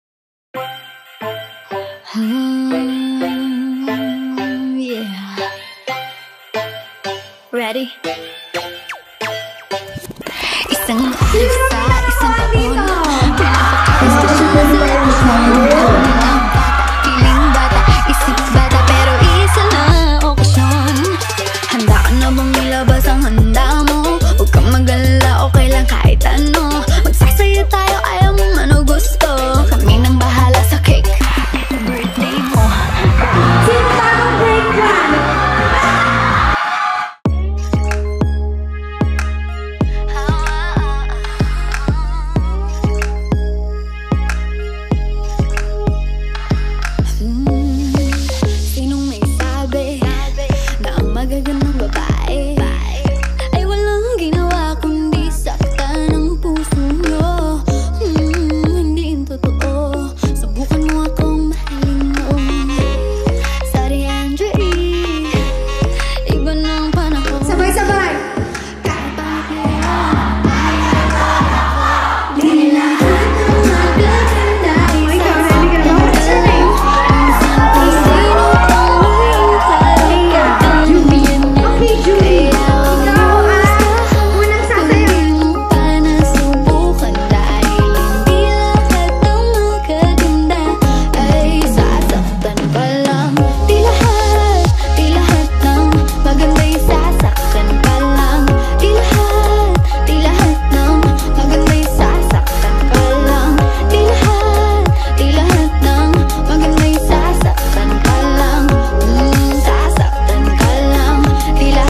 Donnalyn Bartolome Suprise Mall Tour at SM City Baliwag [Highlights]
Here's the official Video Highlights of "Suprise Mall Tour" at SM City Baliwag!